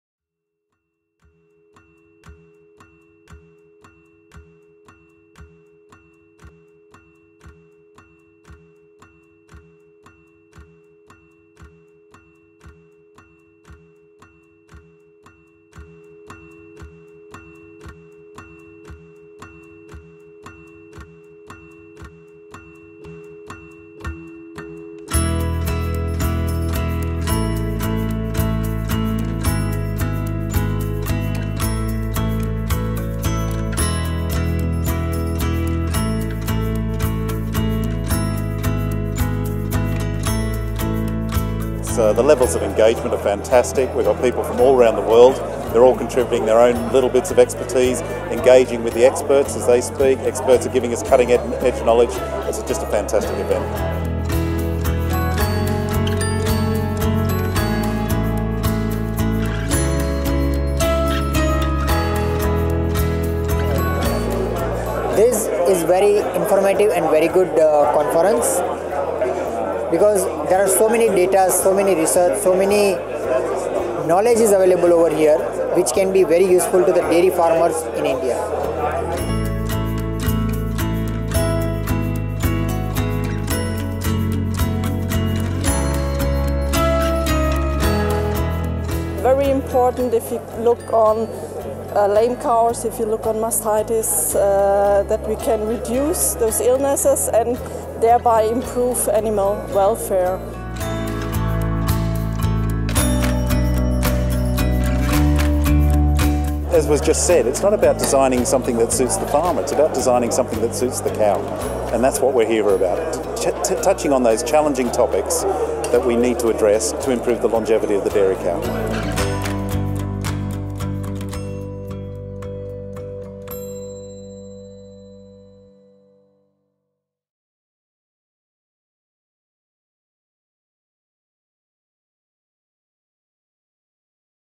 DeLaval Cow Longevity Conference Summary Film
A short film on the Cow Longevity Conference 2013 that was held at Hamra Farm in Sweden. The conference gathered scientists around the world to discuss the shorter productive lifetime of the dairy cow in intensive milk production. The decrease in dairy cow longevity is mainly the result of involuntary culling due to poor health or fertility problems. This can lead not only to lower profitability, but also to concerns from consumers, who are becoming increasingly aware of animal welfare issues.